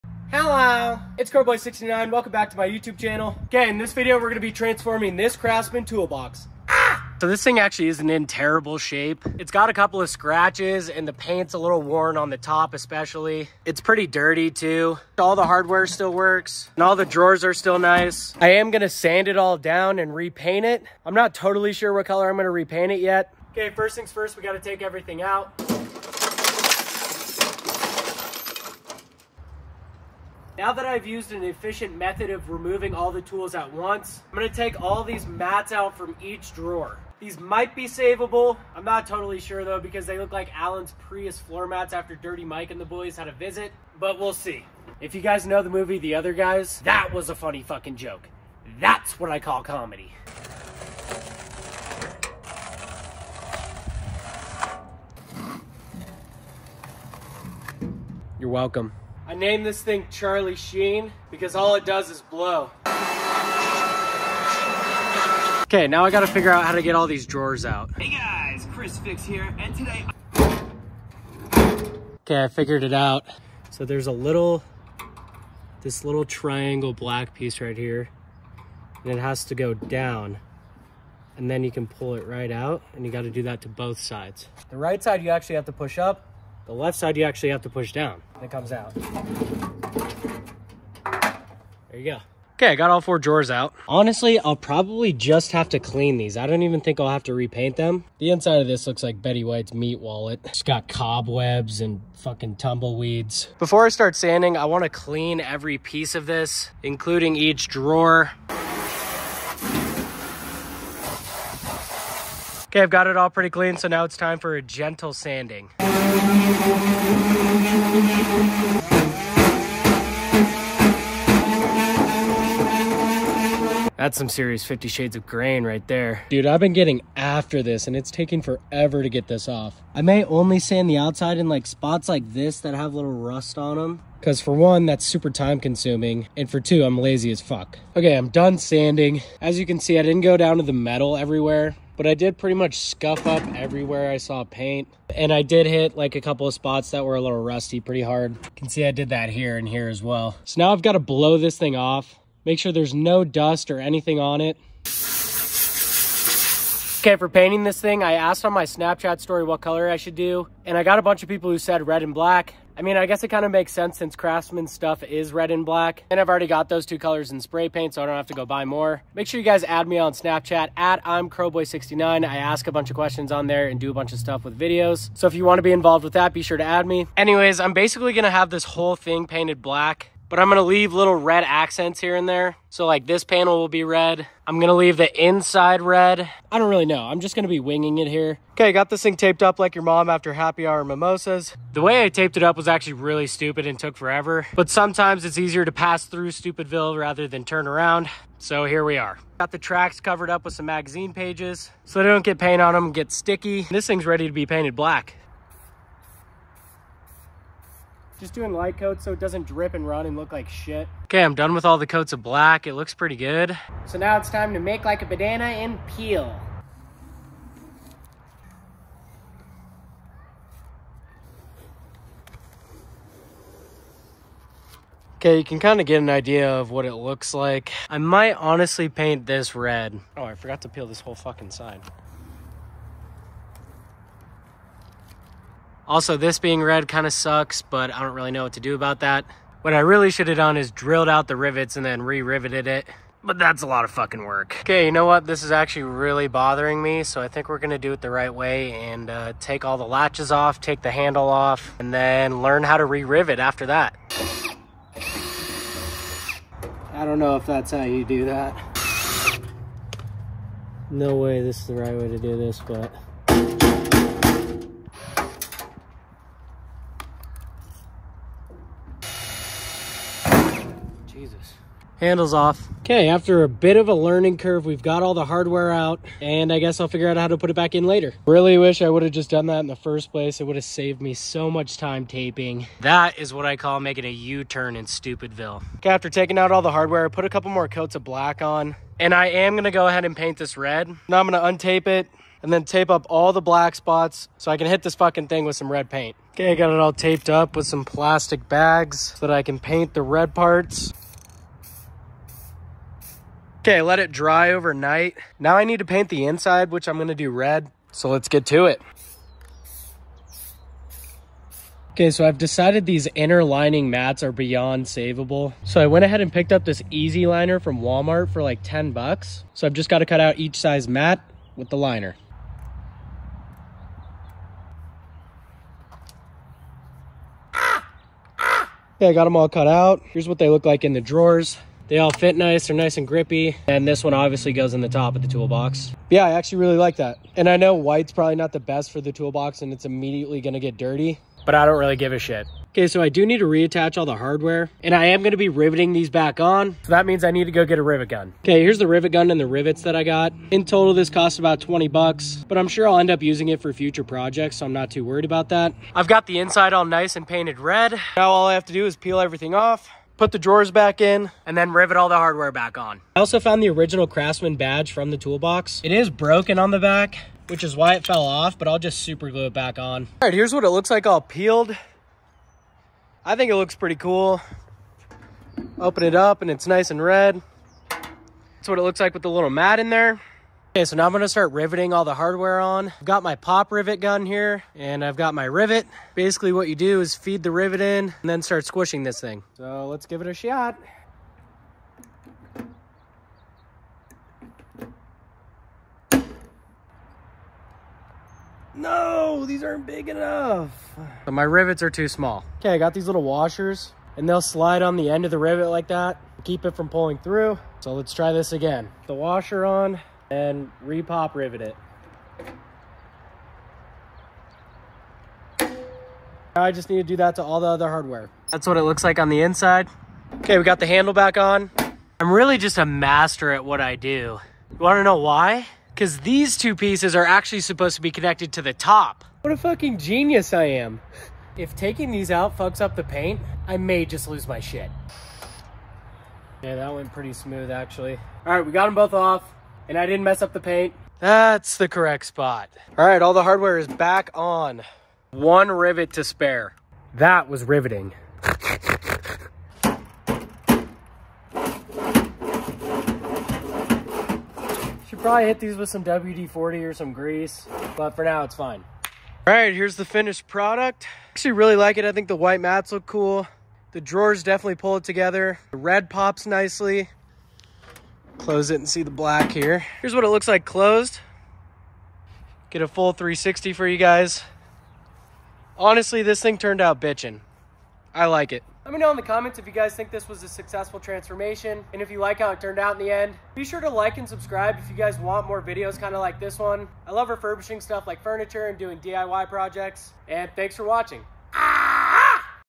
Can I Restore an Old Tool Box?
In this video I attempt to restore an old tool box I have had for years. Let me know how you think it turned out. CAW!